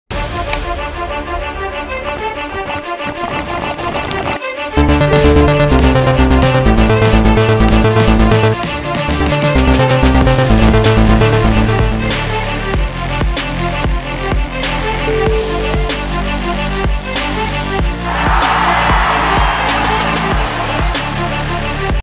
Catching up with the train. Anime characters
Anime hero running to catch up with the train using insane skills
anime animation Animememe anime hero simulator new anime Anime characters react Animeboy